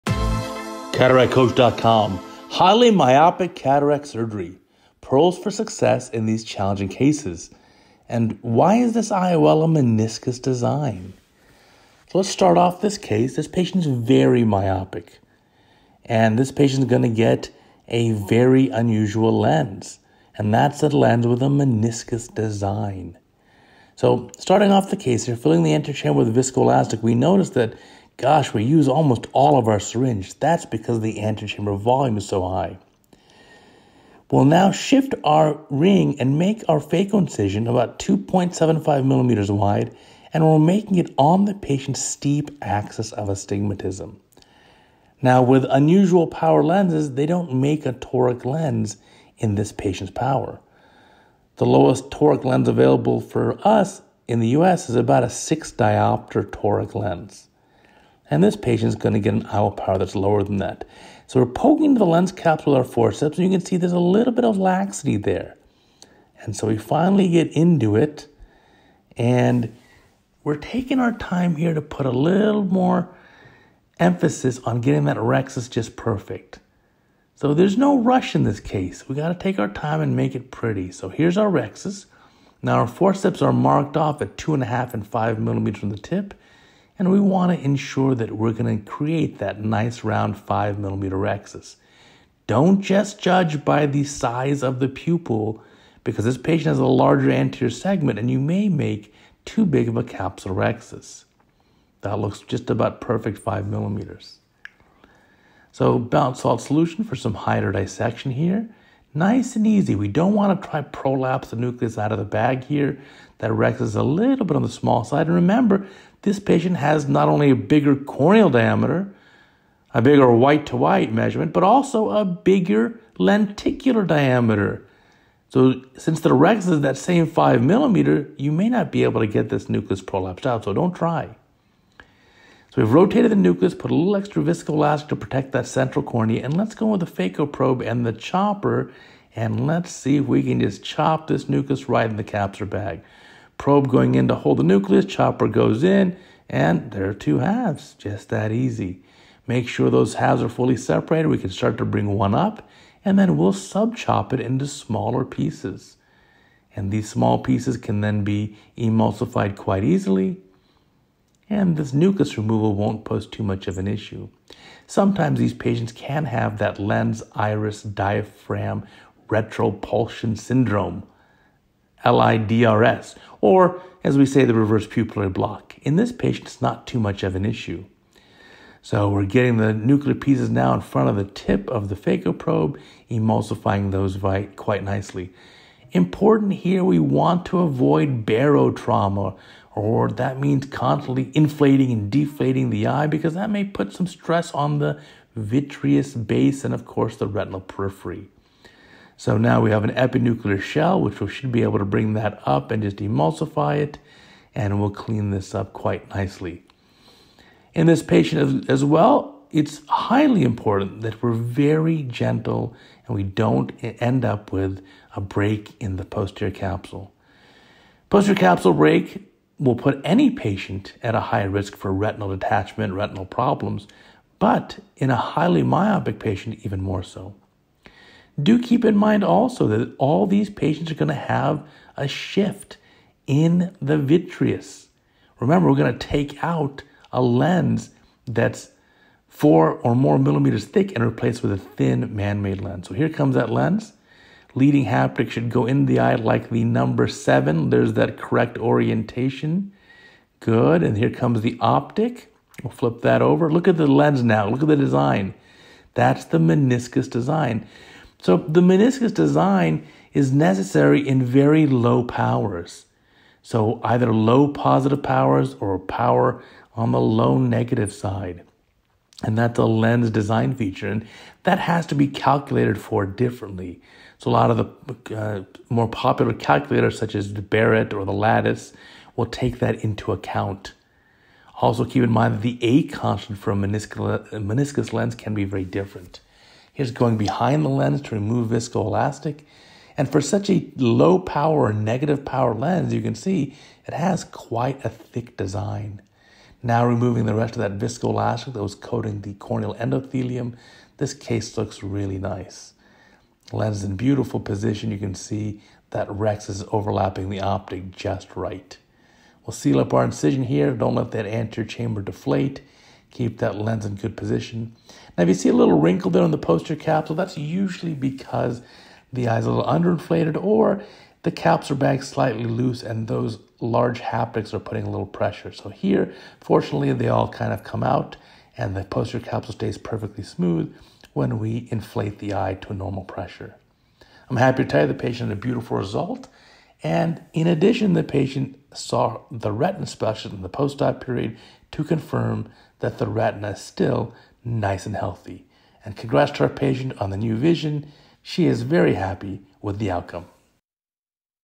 highly myopic patient having cataract surgery
These are some of the most rewarding cataract surgeries to perform since we can correct the cataract as well as the extreme myopia at the same time. These patients are often -15 diopters, -20 diopters, or even more myopic and while they often wish for perfect distance vision, we must be careful to land on the side of mild myopia instead of mild hyperopia. The surgery itself is also more challenging because of issues such as reverse pupillary block (also called LIDRS: lens iris diaphragm retropulsion syndrome). In addition, barotrauma from changes in pressure may put stress on the vitreous base and therefore traction on the retina. These patients should have their retinas checked thoroughly before cataract surgery and then again in the post-operative period.